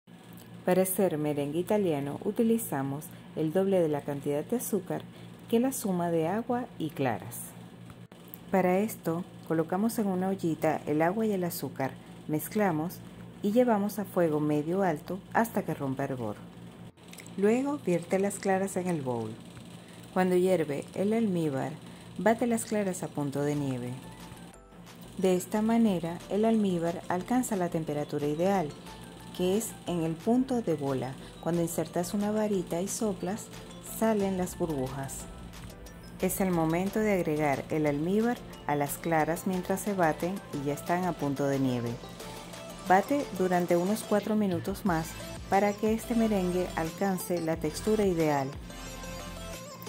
MERENGUE ITALIANO
Fórmula: la suma de agua y claras es el total de azúcar a utilizar.

Ingredientes:
80 gr de agua
80 gr de claras
160 gr de azúcar
Esencia (opcional)

Truco para todos los merengues: agrega al final unas gotas de limón, esto le da mejor textura y más firmeza.

VIDEOS RECOMENDADOS

Yusmely Recetas / Recetas en Casa

Recetas en Casa

Recetas en Casa